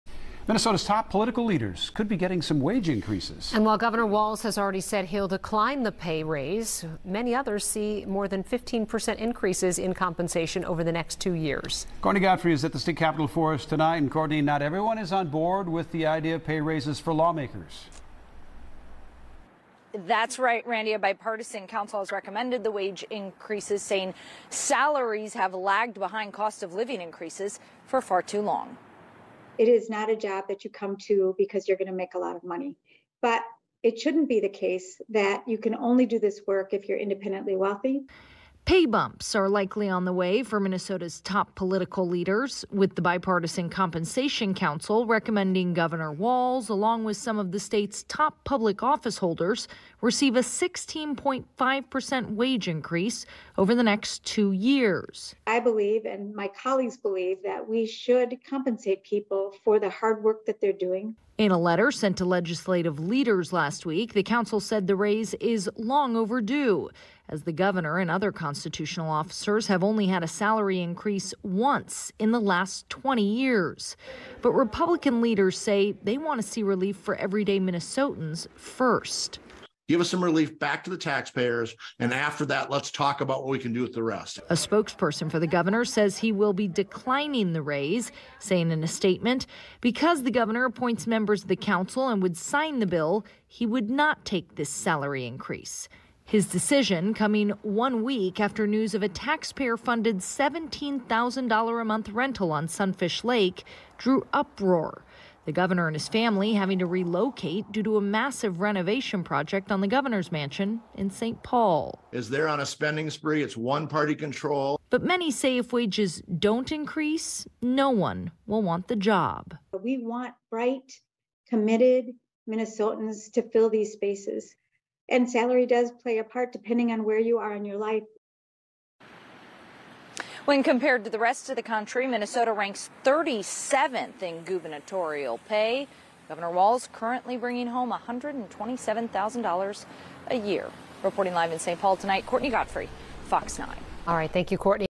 State lawmakers due pay increases I KMSP FOX 9
Some lawmakers could see 15% pay increases over the next two years as part of raises due to the Minnesota Legislature.